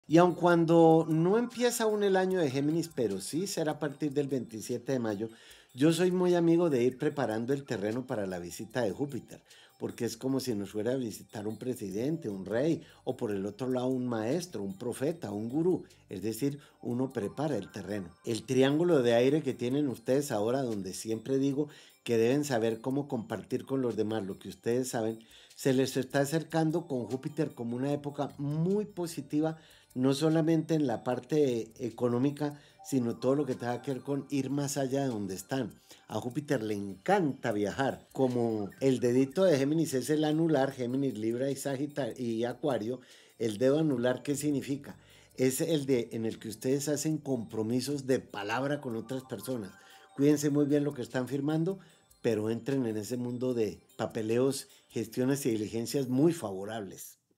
♊ Géminis del 12 al 18 de Feb | Júpiter en Géminis 2024: ¡Cambios Positivos! 🌟
Júpiter en Géminis 2024: ¡Prepárate para Cambios Positivos! 🌟 en @CodigosTV

¡Géminis, se acerca el año de Júpiter a partir del 27 de mayo! Prepárate para una época positiva de oportunidades económicas, viajes y compromisos. Descubre más sobre cómo aprovechar esta energía en el video.

Palabras clave y SEO: Júpiter en Géminis, Cambios Positivos, Oportunidades Económicas, Viajes, Compromisos, Astrología, Horóscopo, Consejos Astrológicos.

Reserva tu consulta astrológica con Mauricio Puerta en el sitio web